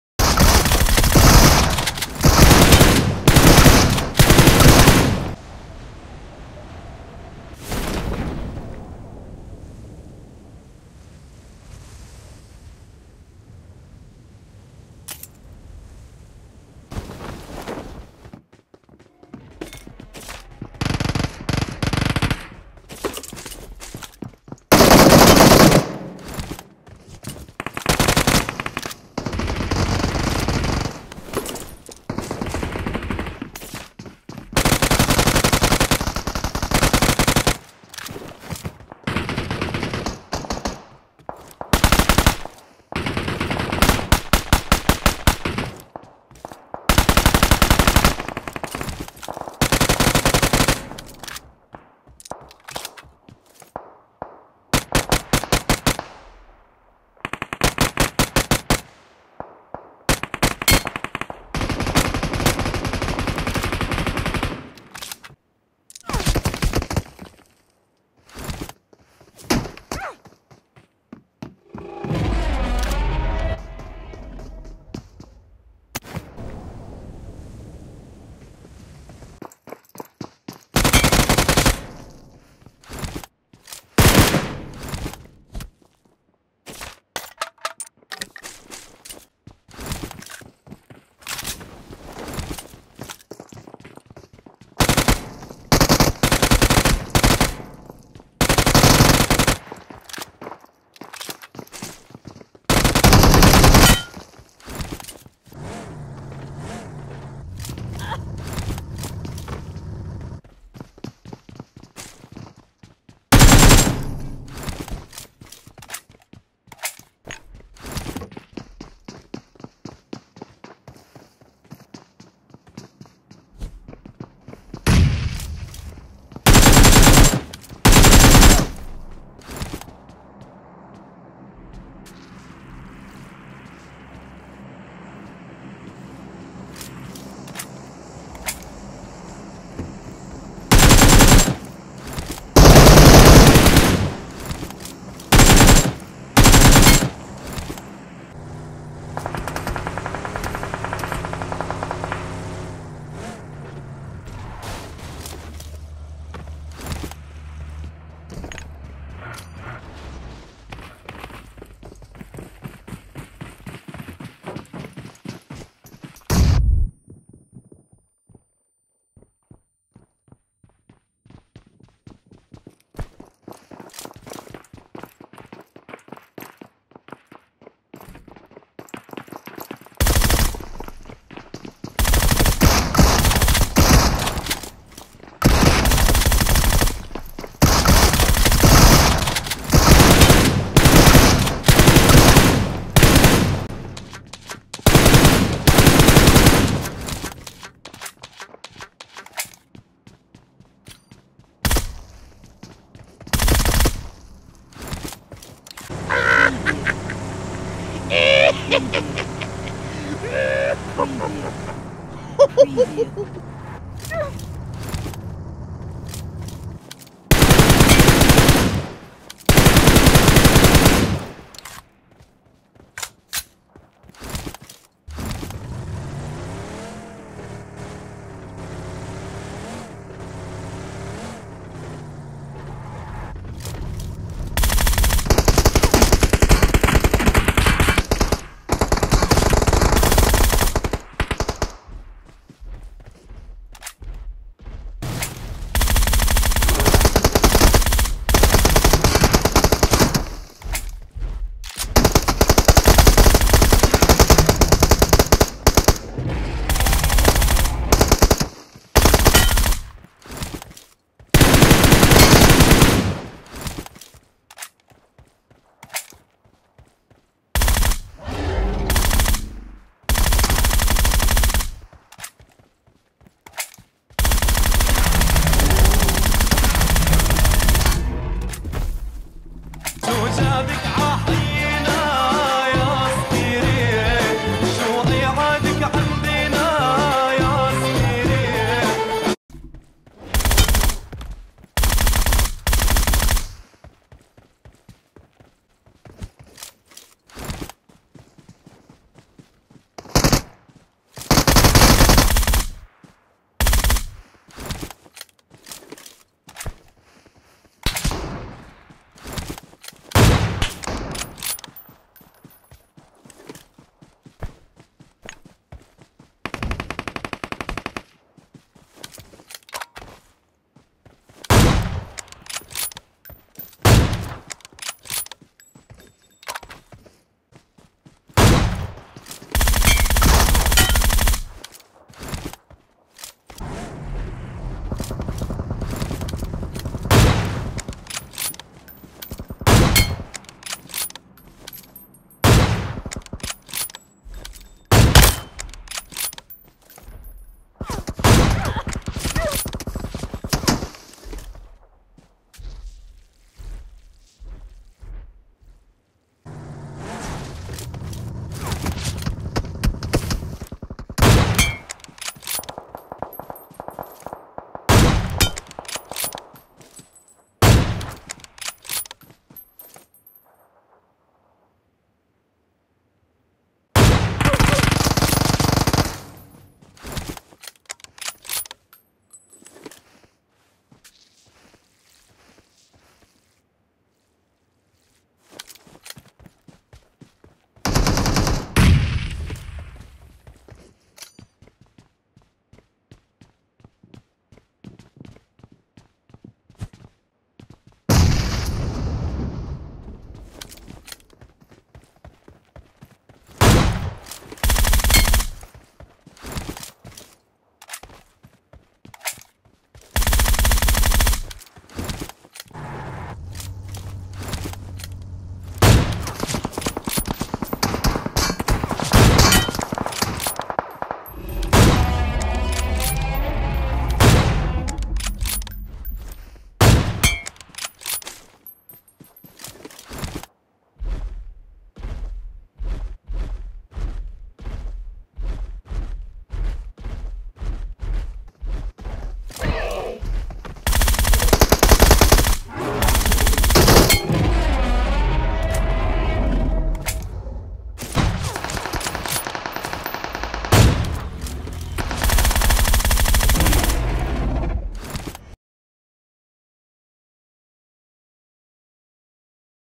34 KILLS solo vs squad.. INSANE SHOTS!
34 kills solo vs squad in NEW DINOSAUR mode.. Insane shots